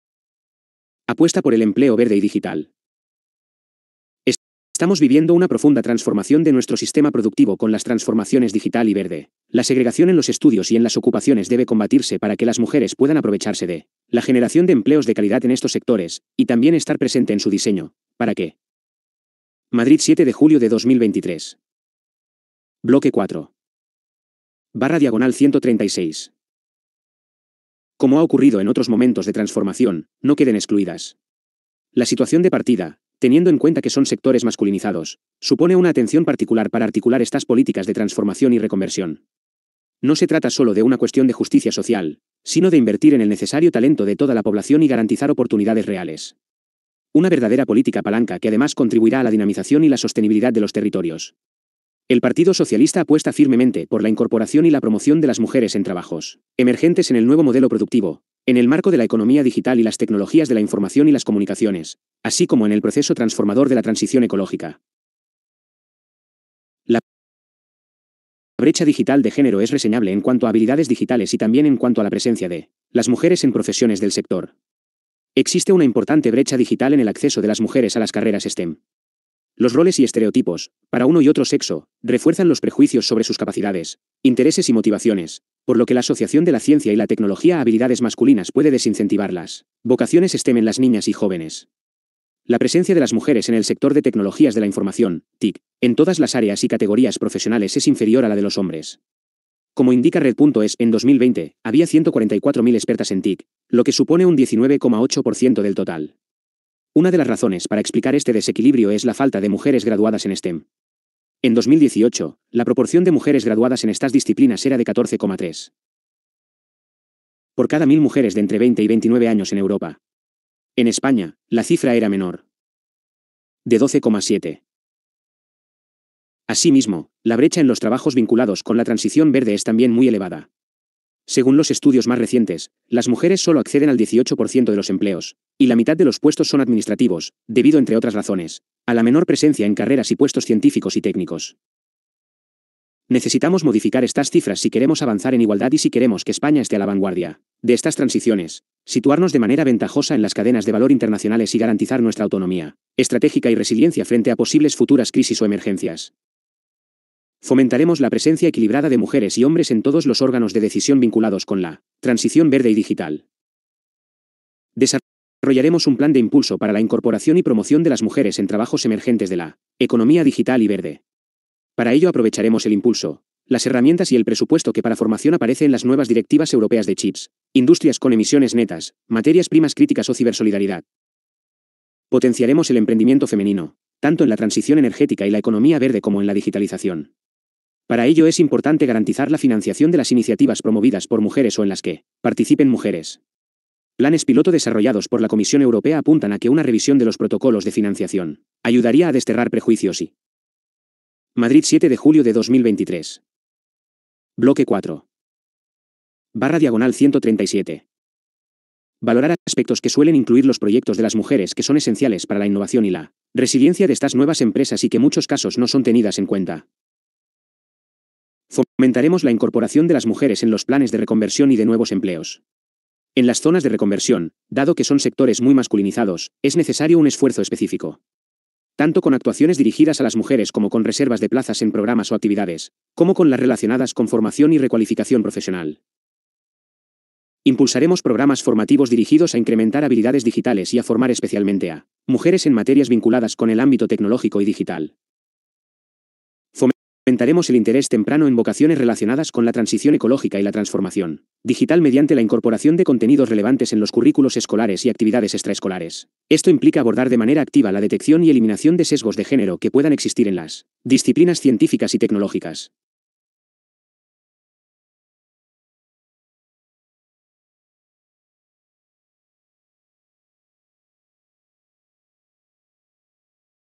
BLOQUE IV: EMPLEO VERDE - Lectura del programa de PSOE 2023 - pag. 135 a 137
Lectura del programma de PSOE

Más información:
Página web: Spainbelike.com